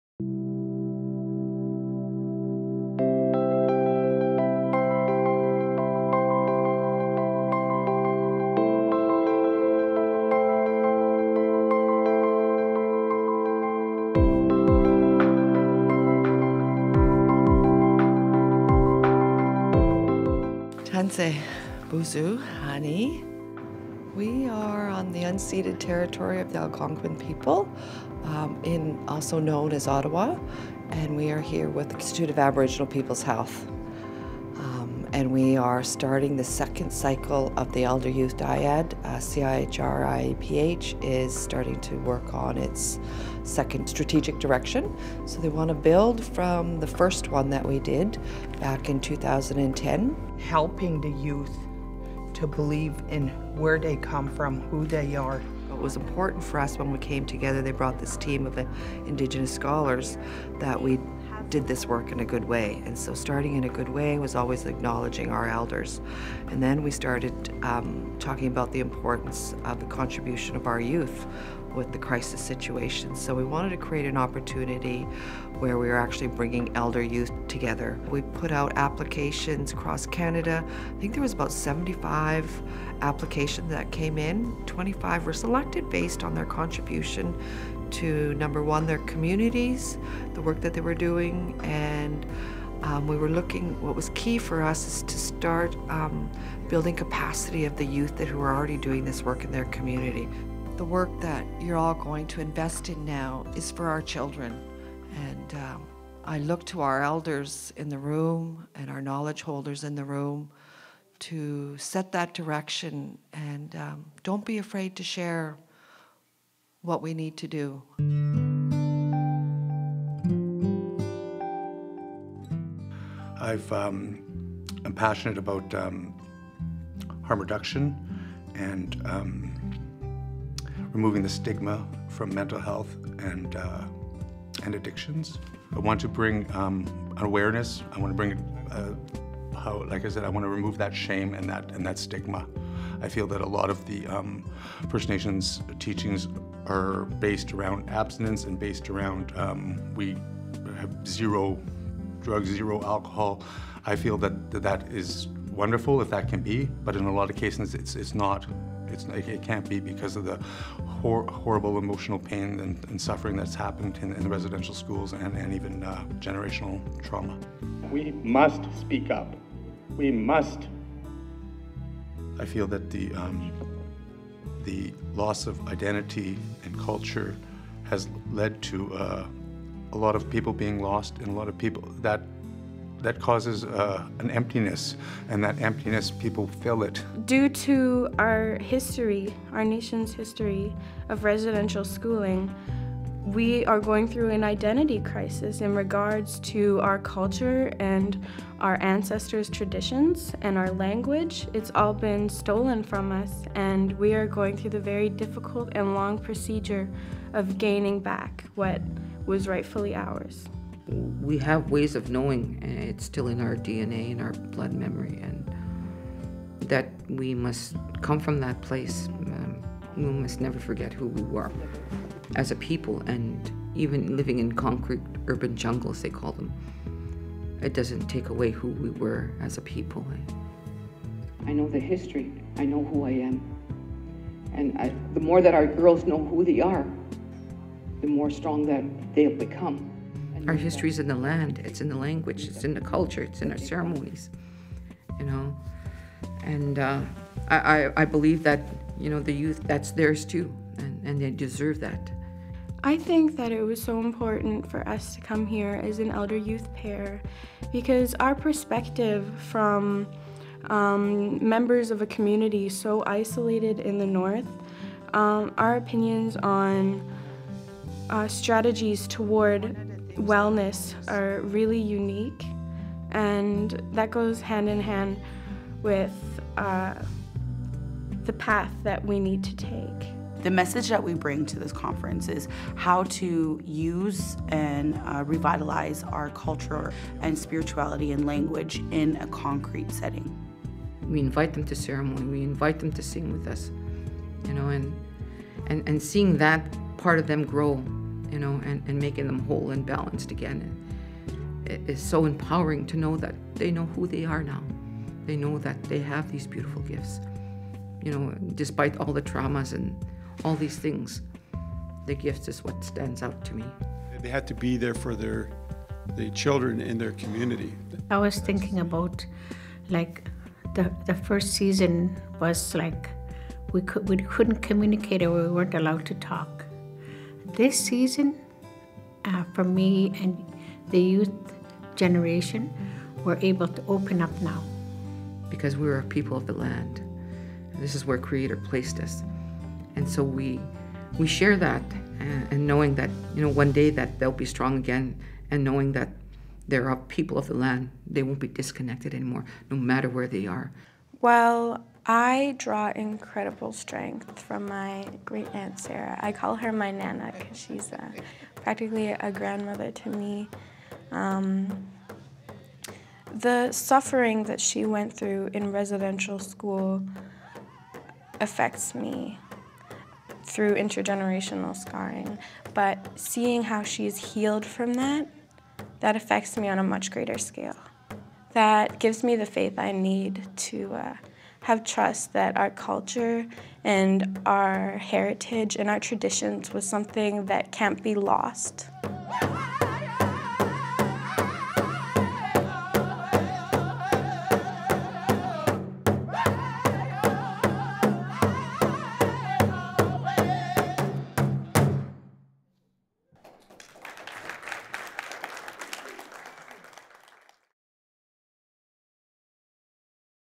CIHR-IAPH 2017 Elder-Youth Dialogue on Indigenous Health Priorities
The Elder-Youth Dialogue, hosted by the Institute of Aboriginal Peoples' Health (IAPH) at the Canadian Institutes of Health Research (CIHR) was held during March 12-15, 2017, on the unceded and traditional territories of the Algonquin Peoples.

The Dialogue brought together 25 Indigenous Elder-youth pairs from across Canada to share with IAPH, their research, health, and wellness priorities across five thematic areas: health priorities from a community perspective; Missing and Murdered Indigenous Women, Girls, and Two Spirited Peoples; Truth and Reconciliation; health and wellness; and education.